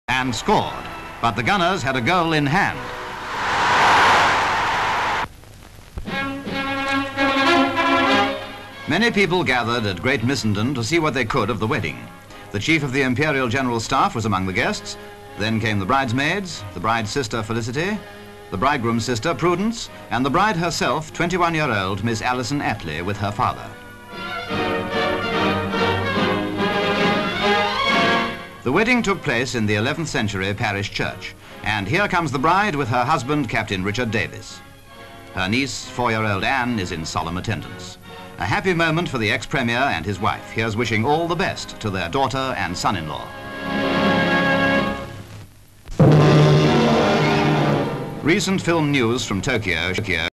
ATTLEE'S DAUGHTER WEDS
(13 Mar 1952) Many people gathered at Great Missenden to see what they could of the wedding of 21-year-old Miss Alison Attlee and Captain Richard Davis.